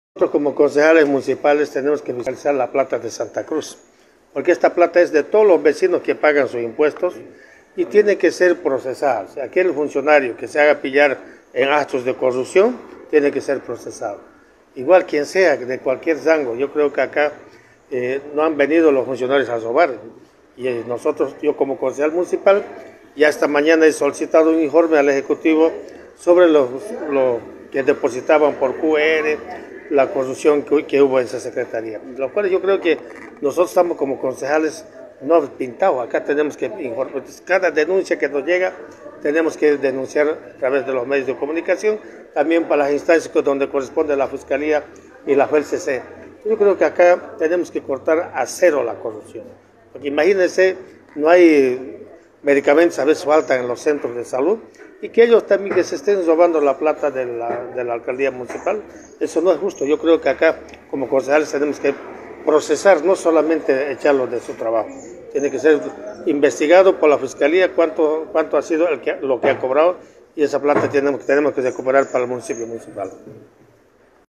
MAS pide procesar a cualquier funcionario que "se deje pillar en actos de corrupción" al referirs
MAS pide procesar a cualquier funcionario que "se deje pillar en actos de corrupción" al referirse a la ex directora del Jardín botánico Andrea Forfori

El concejal del MAS, José Quiroz,  señaló que aquel funcionario que sea hallado en actos de corrupción debe ser procesado sin importar el rango que ocupen, por ello solicito un informe al ejecutivo sobre los depósitos por QR en la dirección del jardín botánico, pide cortar la corrupción y no es suficiente la destitución.